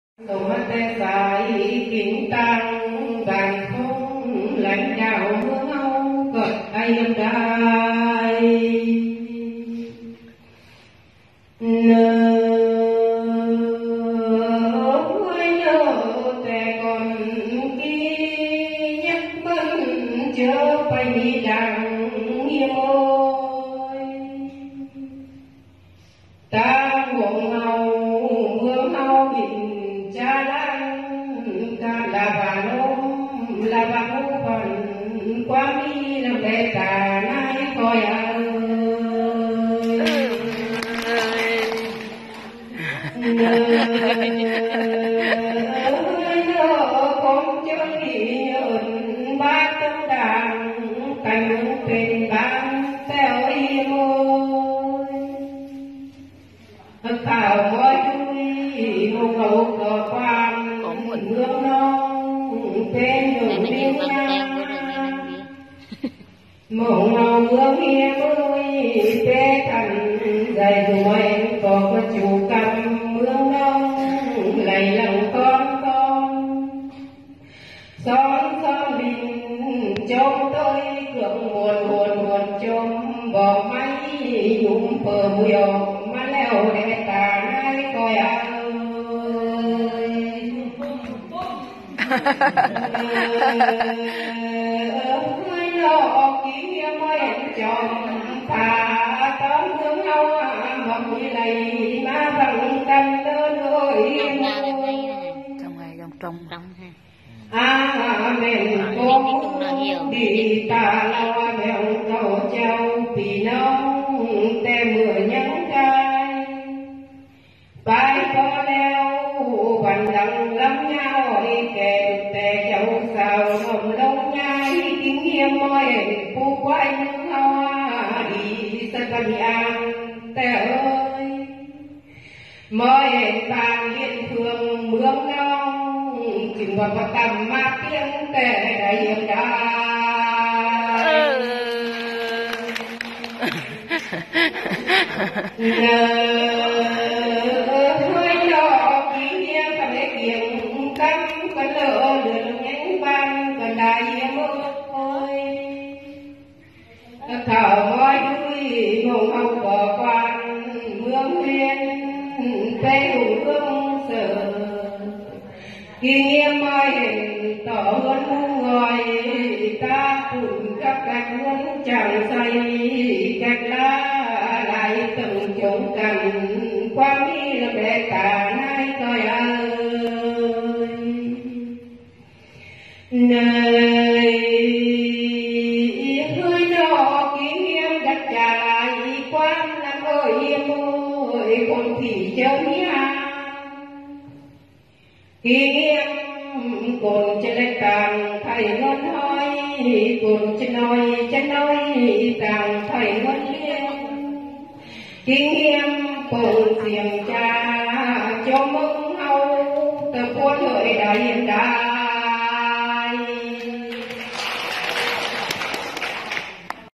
hát nhuôn dân tộc thái nghệ an
Bài hát Nhuôn gìn giữ nét văn hóa thái, giao lưu tiếng thái với bà con người thái Tây Bắc việt nam cũng như tiếng Lào & Thái Lan. mong được mọi người chia sẻ.